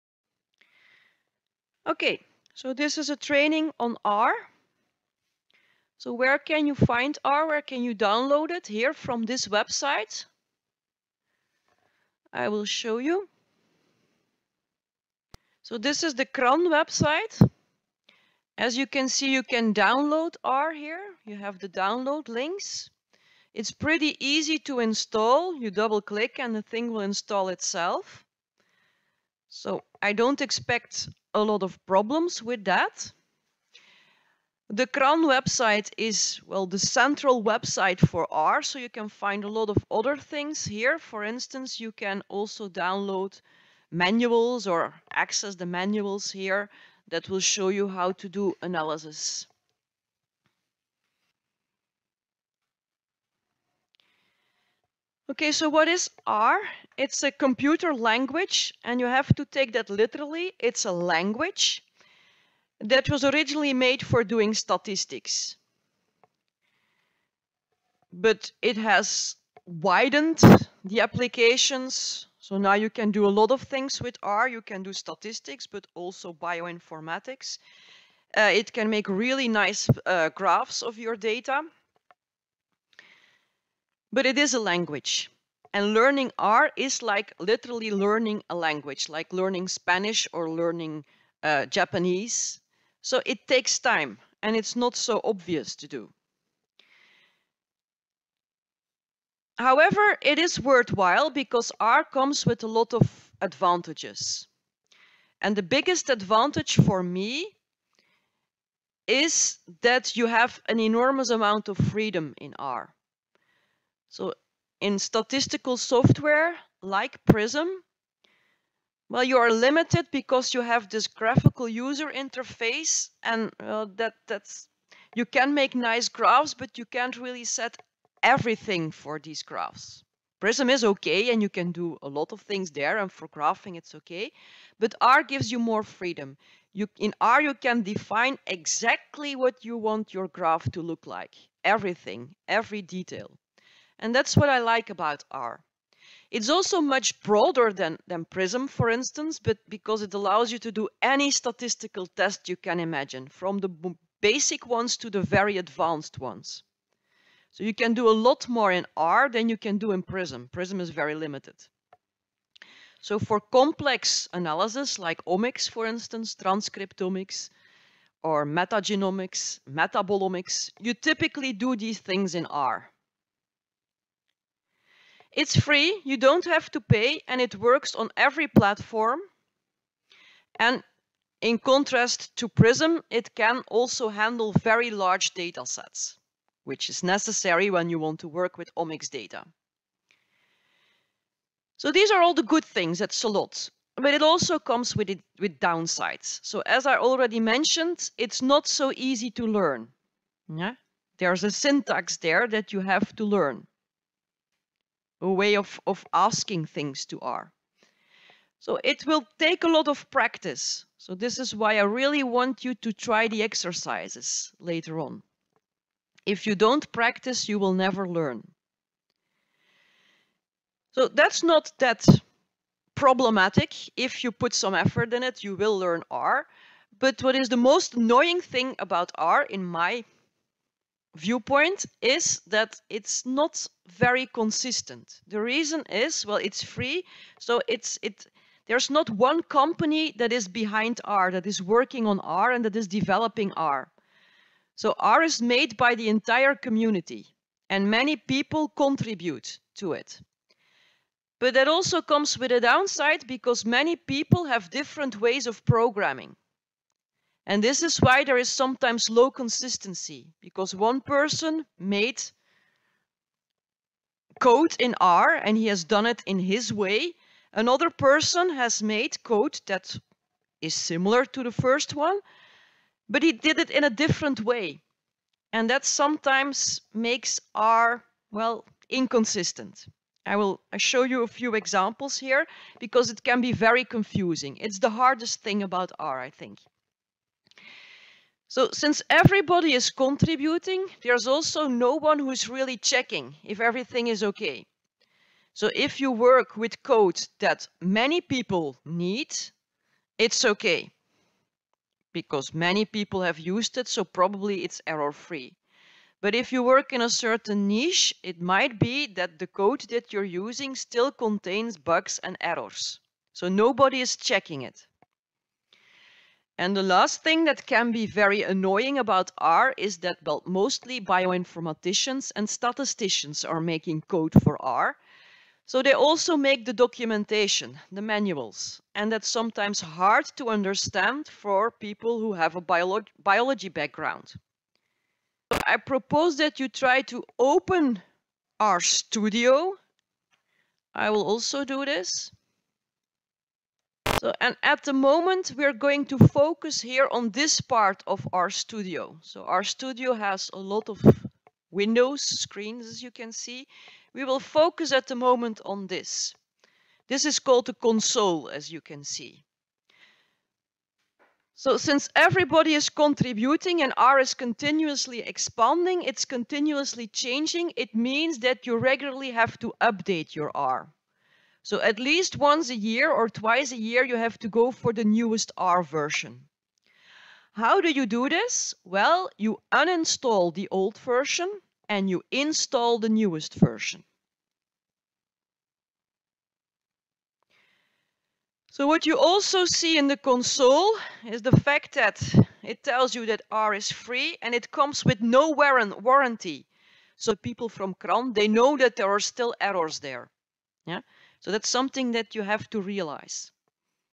R introduction 1 by VIB Bioinformatics Core
What is R?
Where to get it?
Pros and cons of R